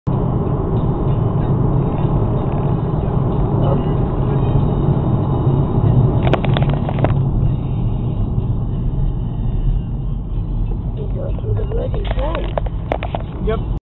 accident A467 (Risca By-Pass) 17/12/18
Vauxhall Vectra crashed through central barrier